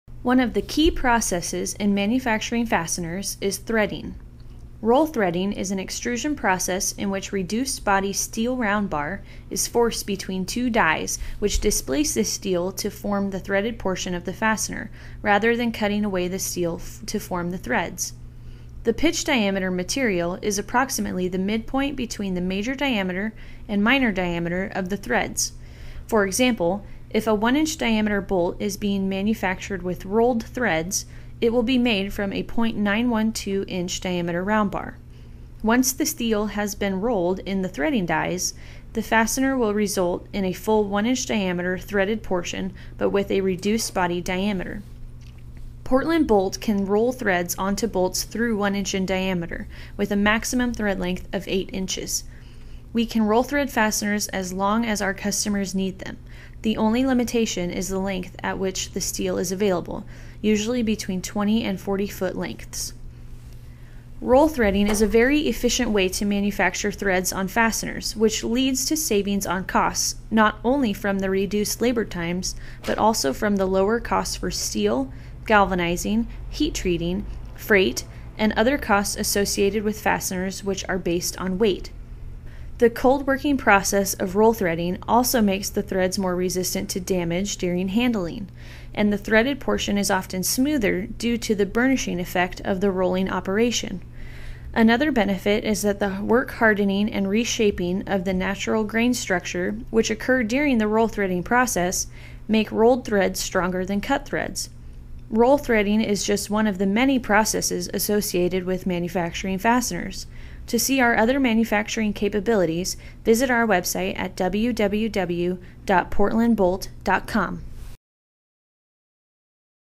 Roll Threading Bolts and Fasteners
Roll threading is a very efficient method of manufacturing threads on bolts and other fasteners. Learn the benefits and limitations of this popular technique from one of the experts at Portland Bolt: Kailey Tillman.